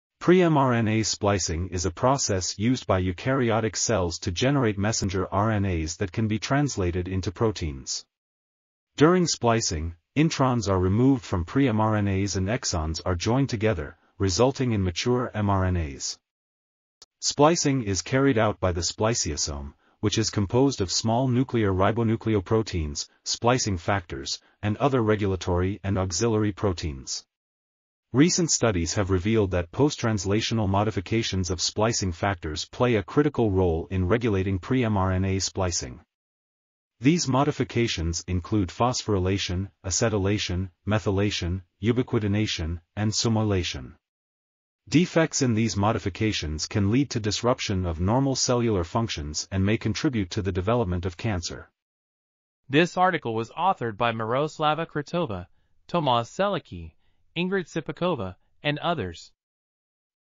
Regulation of Pre-mRNA Splicing: Indispensable Role of Post-Translational Modificatio... | RTCL.TV
### Keywords ###

### Article Attribution ###
Title: Regulation of Pre-mRNA Splicing: Indispensable Role of Post-Translational Modifications of Splicing Factors
Authors: Miroslava Kretova, Tomas Selicky, Ingrid Cipakova ,and Lubos Cipak

### Video Timestamps ###
0:00:00 - Summary
0:00:52 - Title
0:01:00 - End